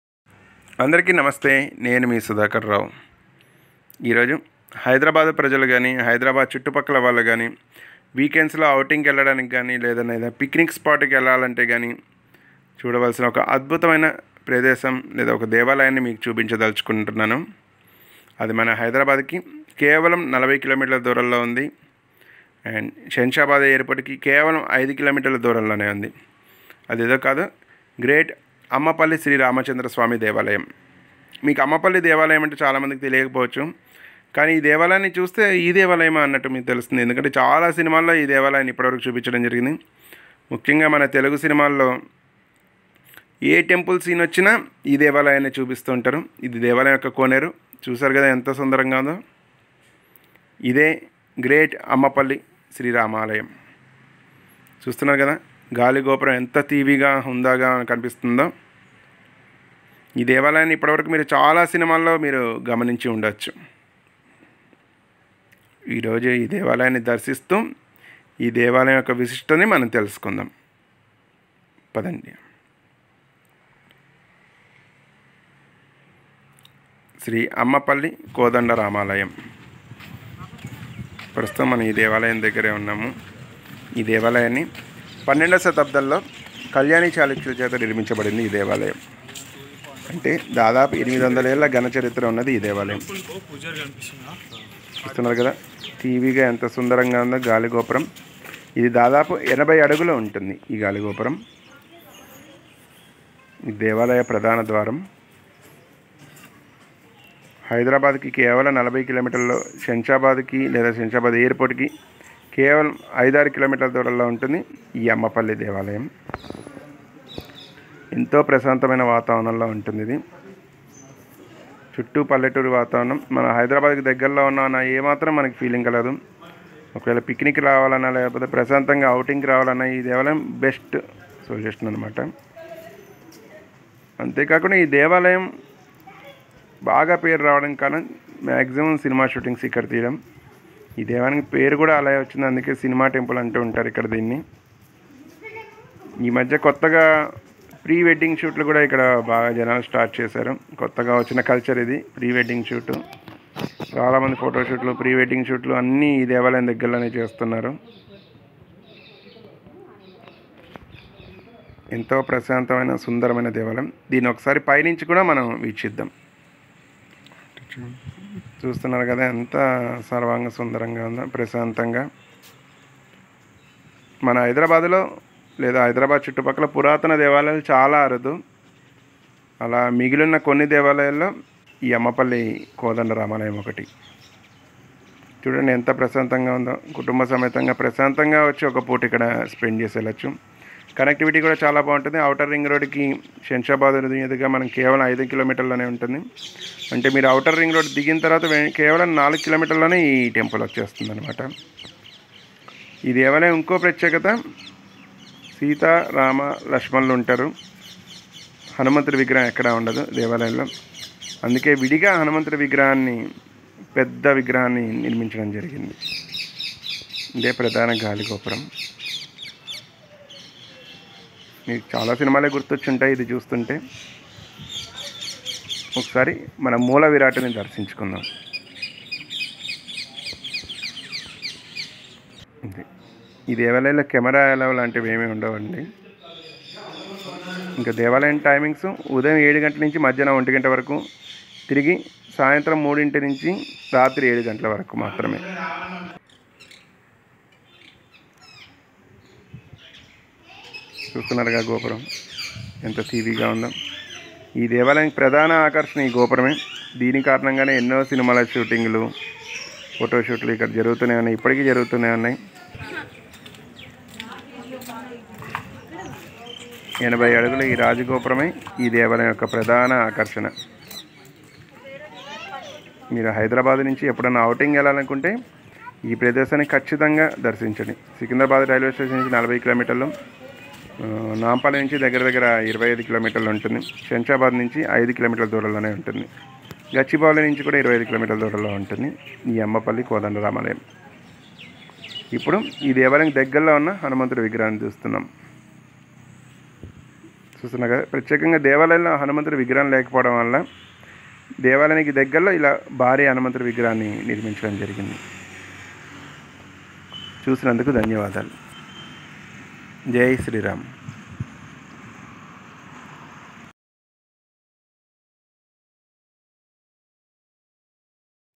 Ammapalli Ramalayam Shamshabad | Pre wedding Shooting Temple | Cinima Gudi
12th century Great Rama Temple, Shamshabad near to Hyderabad